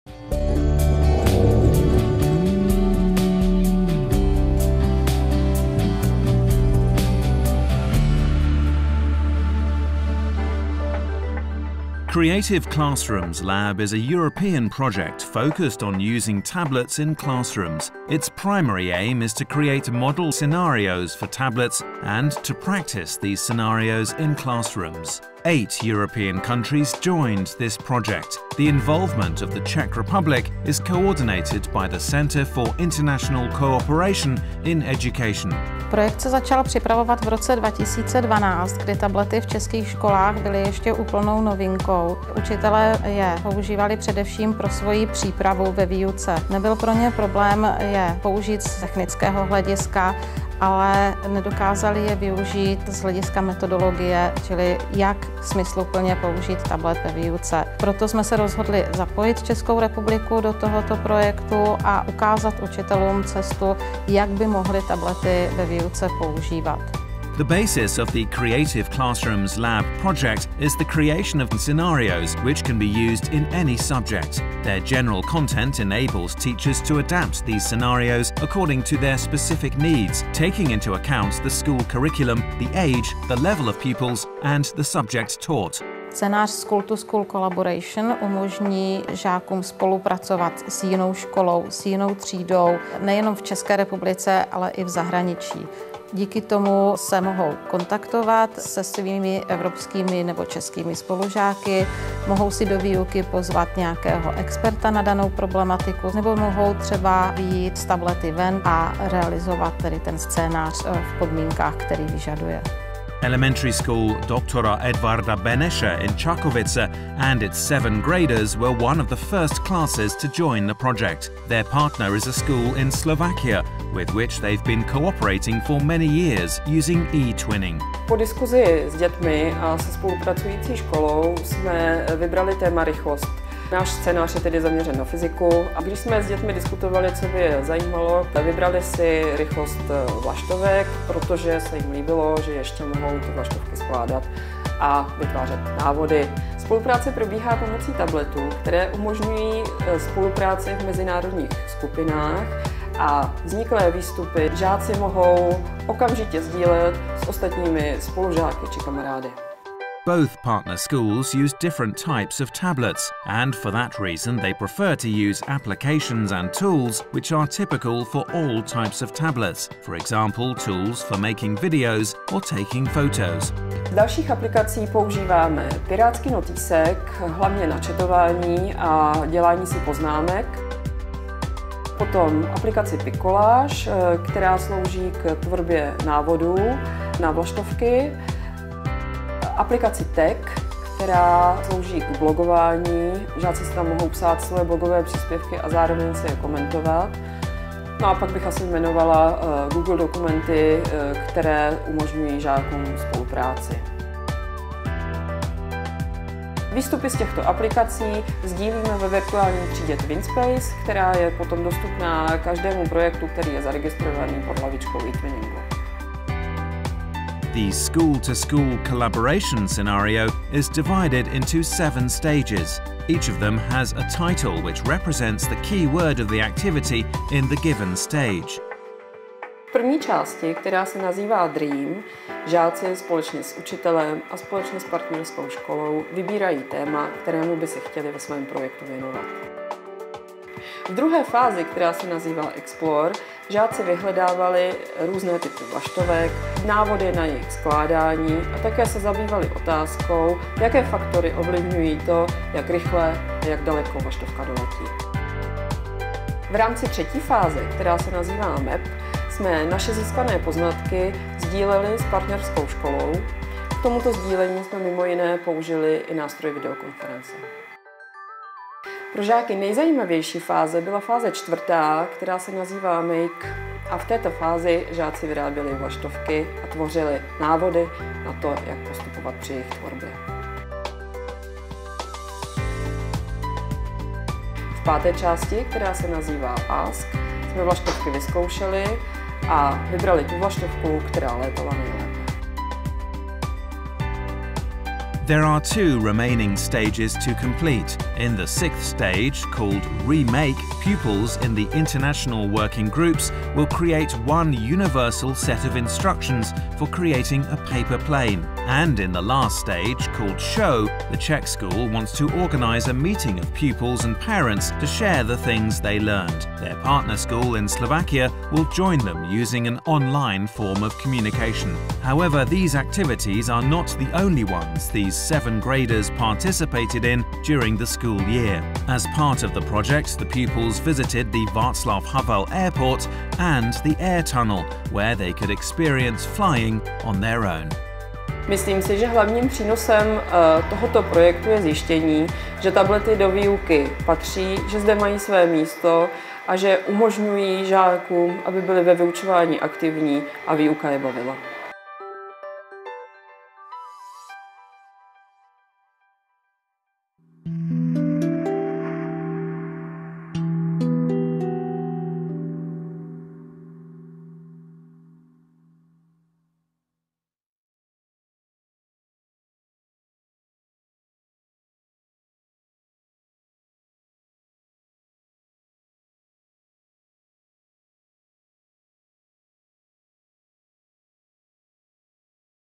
Creative Classrooms Lab - Tablets in schools - Czech Republic
This video is part of a series of Creative Classrooms Lab videos commissioned by nine Ministries of Education featuring schools involved in different tablet policy experimentations.